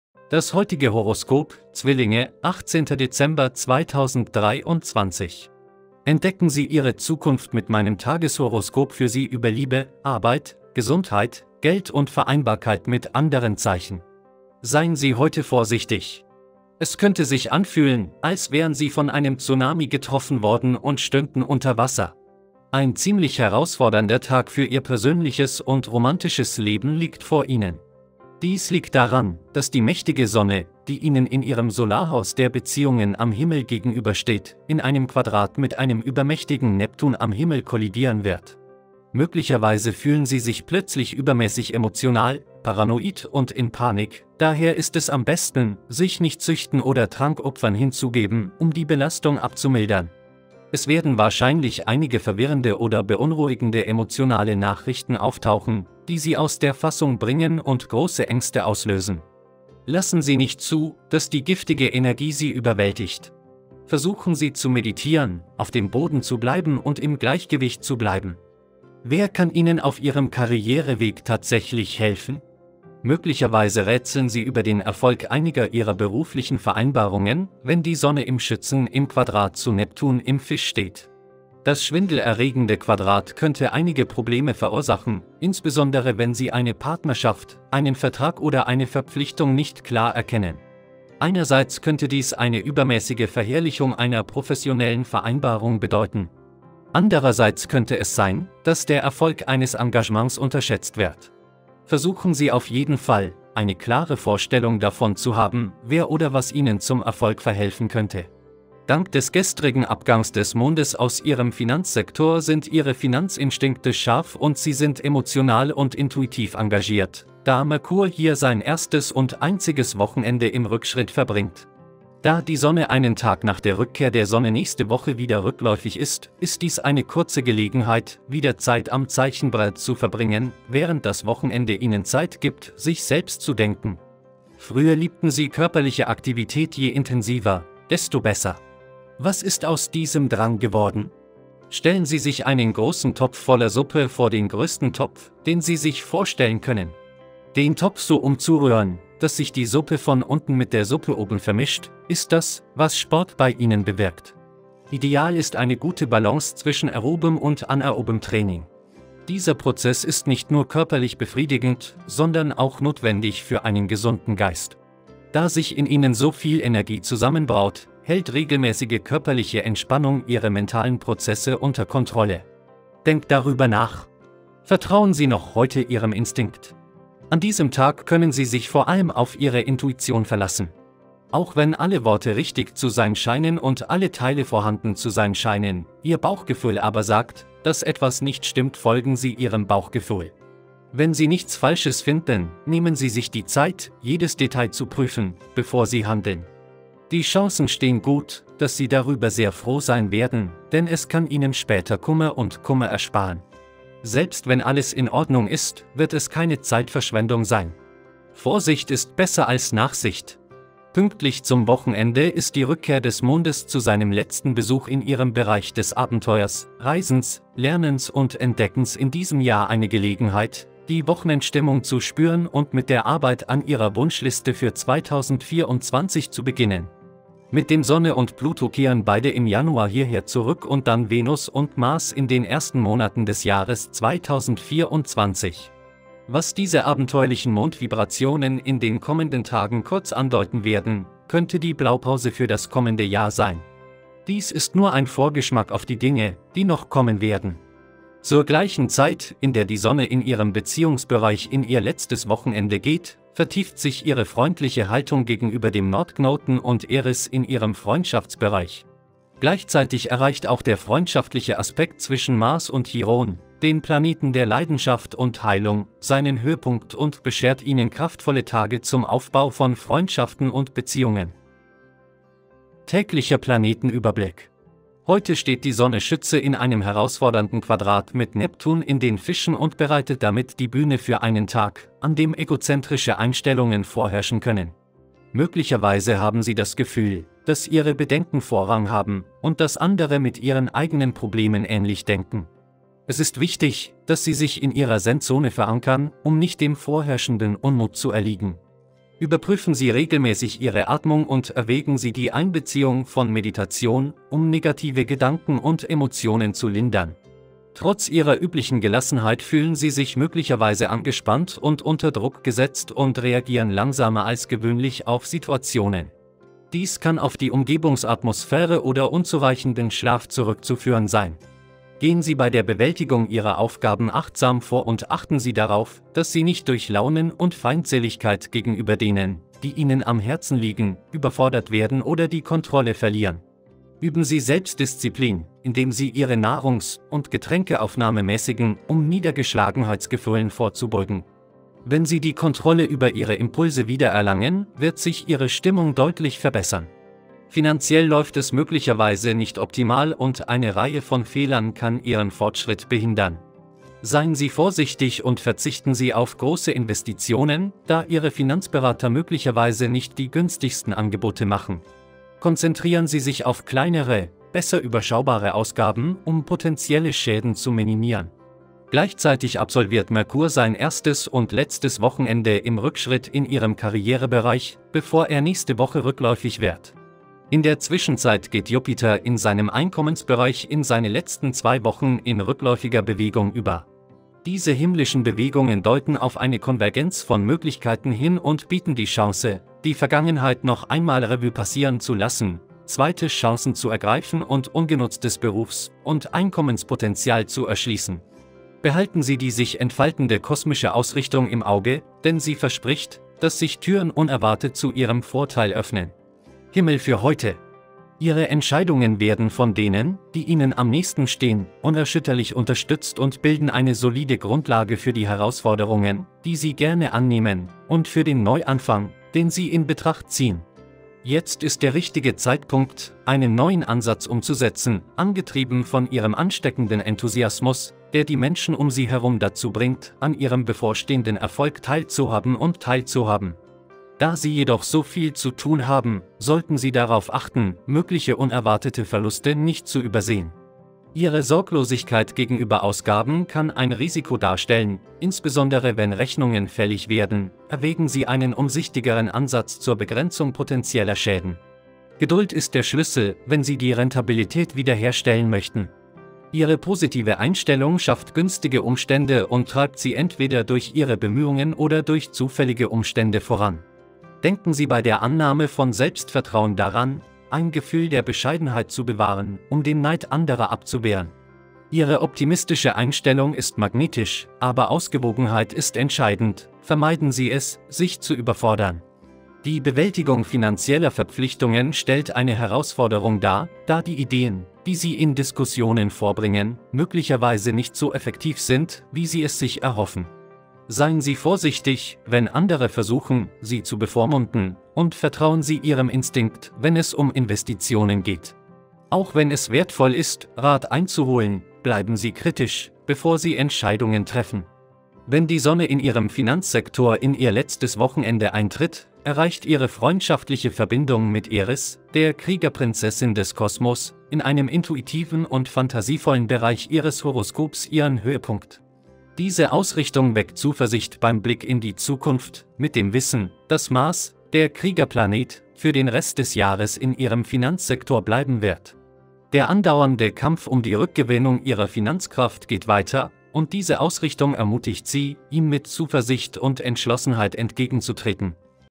😇 EIN WUNDER GESCHIEHT 🙏 ZWILLINGE ♊ HEUTE, 18. DEZEMBER 2023 🔮 Tageshoroskop
Das heutige Horoskop, 18. Dezember 2023. Entdecken Sie Ihre Zukunft mit meinem Tageshoroskop für Sie über Liebe, Arbeit, Gesundheit, Geld und die Verträglichkeit mit anderen Zeichen.